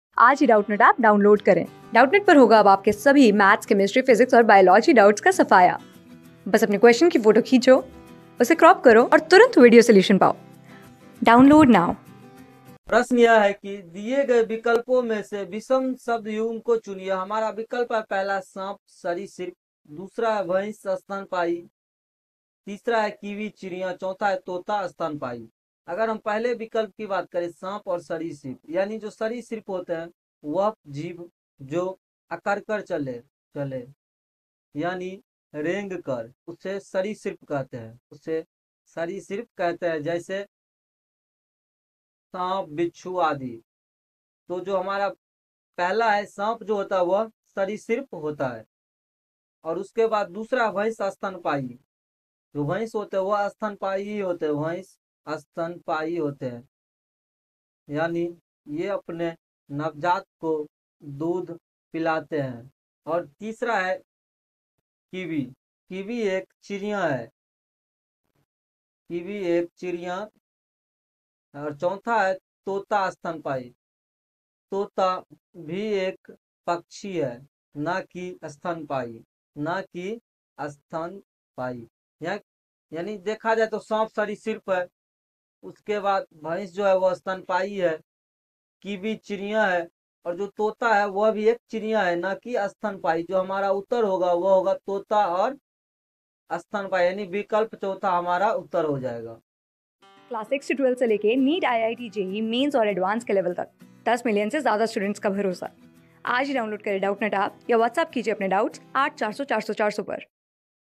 दिए गए विकल्पों में से विषम शब्द युग्म चुनिए : | CLASS 14 | वर्गीकरण | REASONING | Doubtnut
दिए गए विकल्पों में से विषम शब्द युग्म चुनिए :
Class: 14
Subject: REASONING
Chapter: वर्गीकरण
Board:GOVT

👉 Have Any Query? Ask Us.